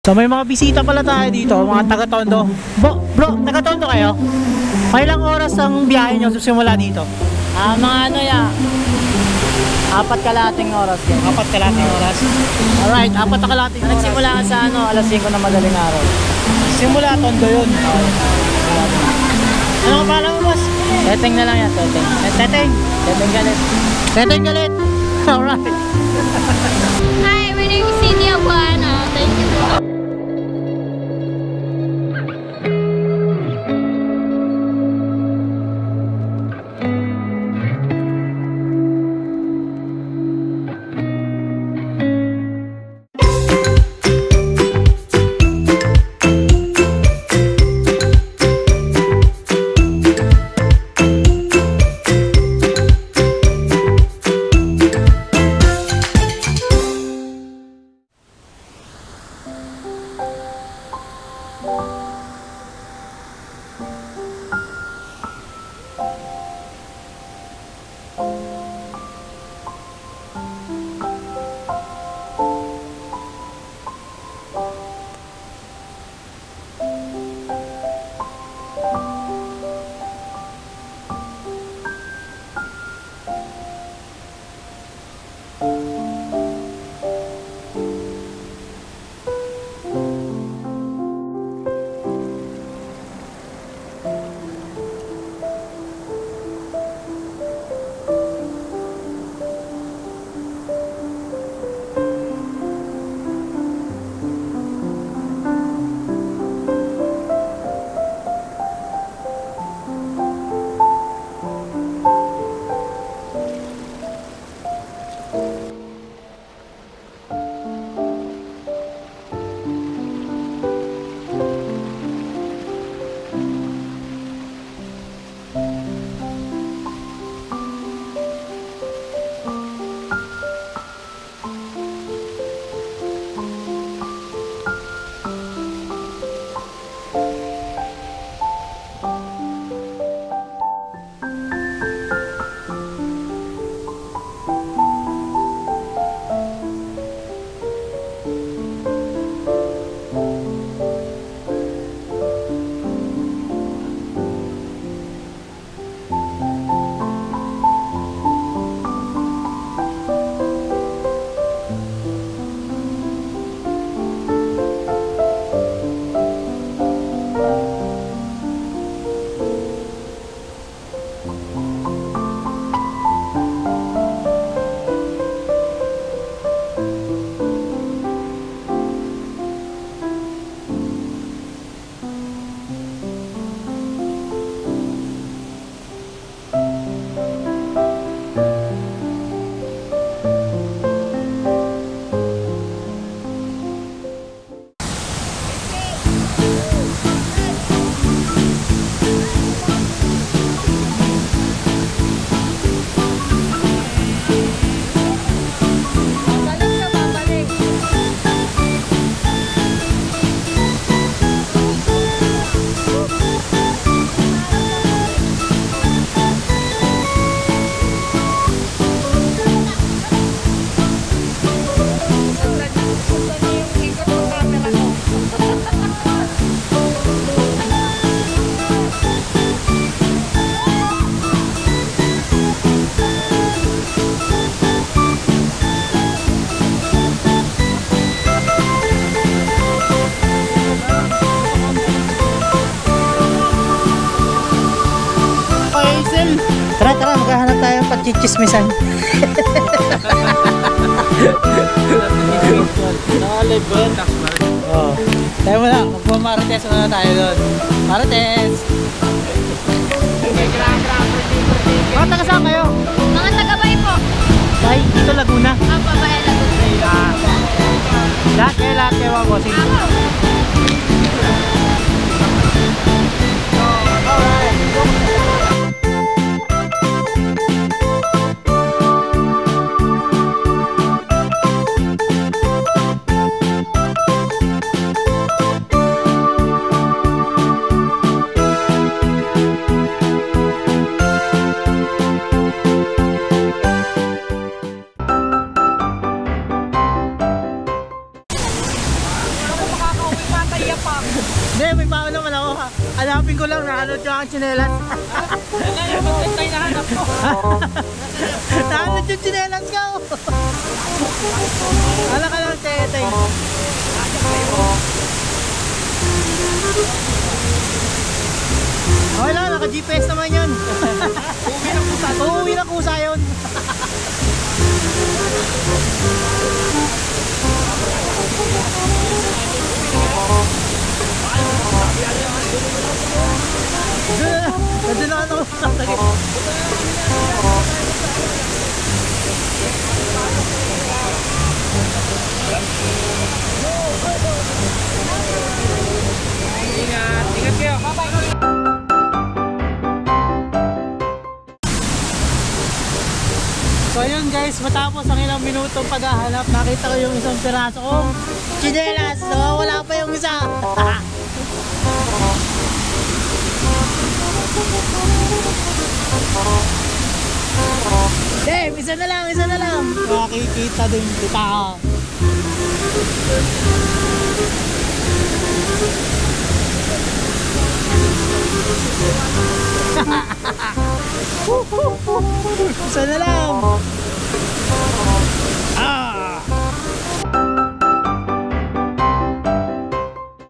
TAYTAY FALLS - CHILL MODE | CS MOTO V50
Chill mode muna tayo dito sa Majayjay, Quezon Prov. - Taytay Falls
Shout-out kay teteng galit from Tondo Manila.
at maraming salamat din kay cindy abuan from Bay, Laguna
at sa mga kasama ko xempre sina Pare at Mare:
Dihlan family, Ram family, Hazel n shai.